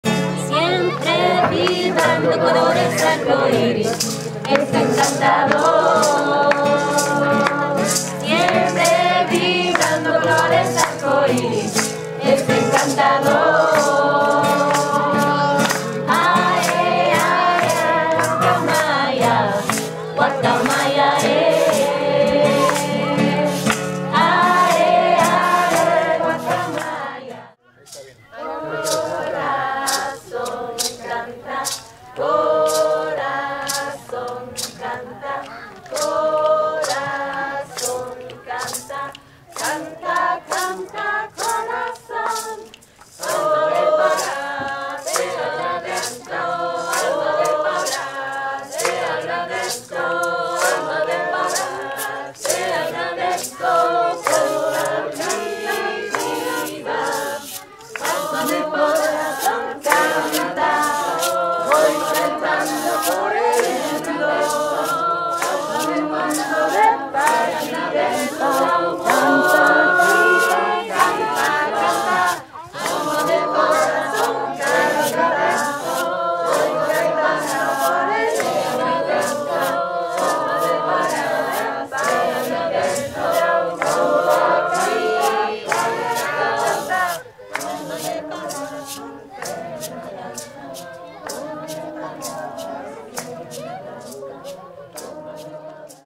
Circulo Vocal en Tepoztlan
Circulo vocal en Tepoztlan - Ven a cantar y improvisar canciones juntos, armonizar sonidos juntos y liberar tu voz en juegos de improvisacion
Imparte: Camilla Chimiak

video by @21pix